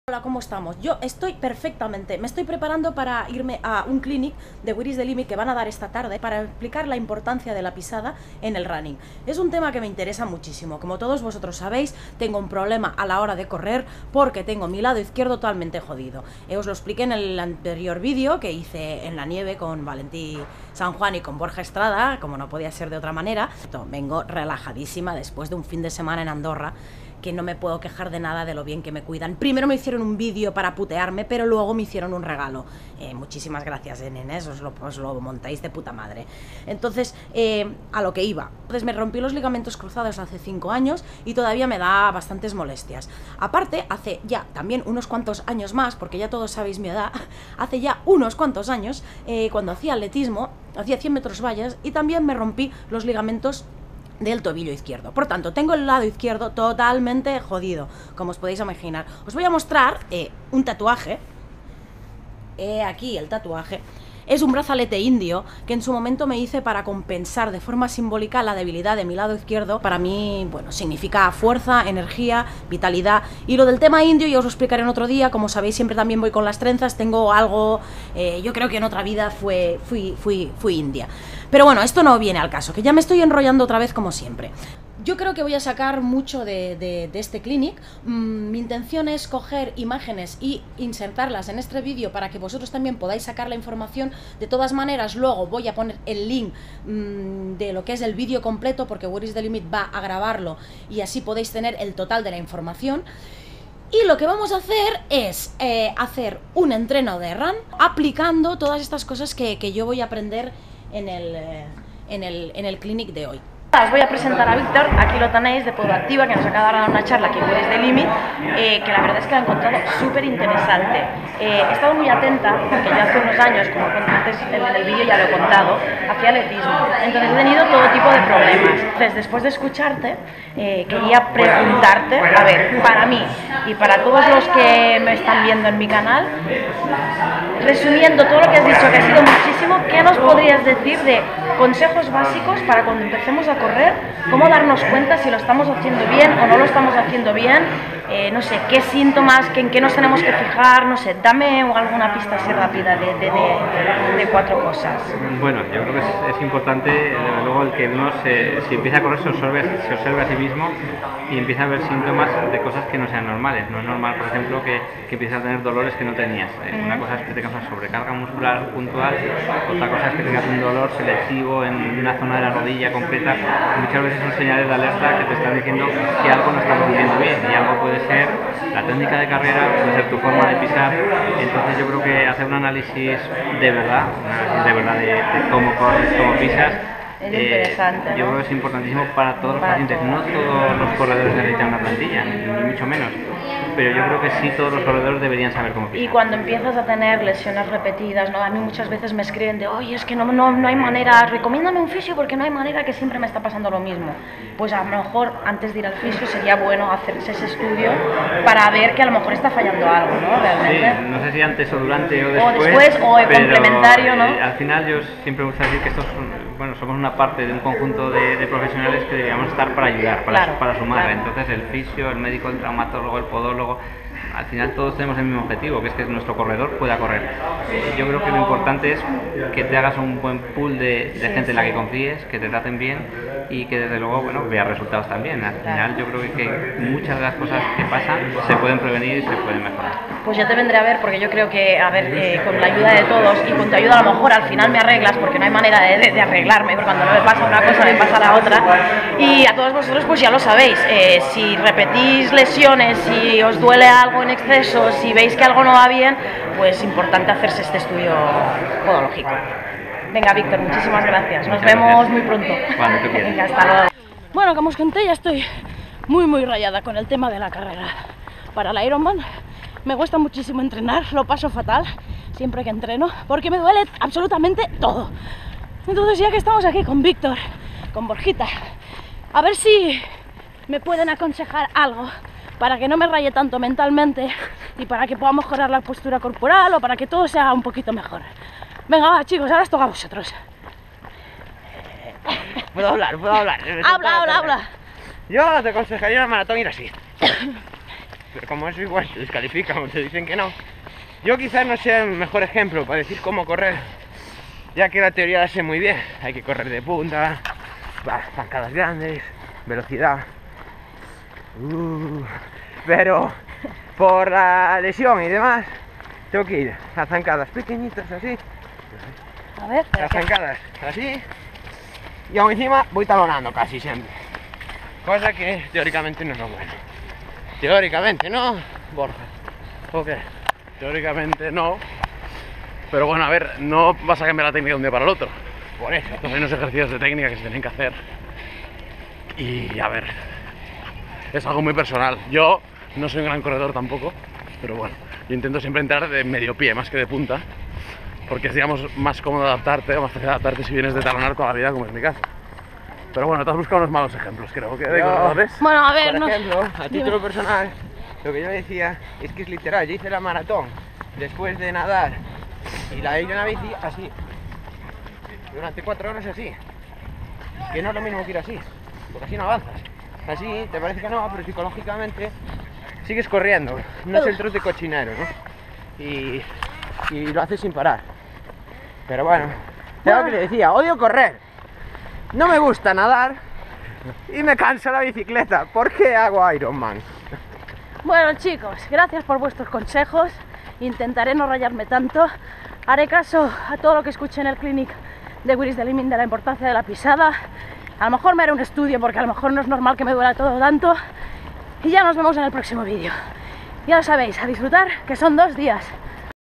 Mis problemas con el running y la importancia de la pisada según @podoactiva en
Lo estoy pasando mal en los entrenos de la carrera. Tengo muchos dolores y eso me afecta a la cabeza. Aquí os lo cuento y lo complemento con las explicaciones del clínic que @whereisthelimit hizo en su sede de Barcelona por gentileza de @podoactiva. Y lo complemento con un entreno de run con @victortasende y @borjaestrada que me dan sus consejos! . Espero que os sirva!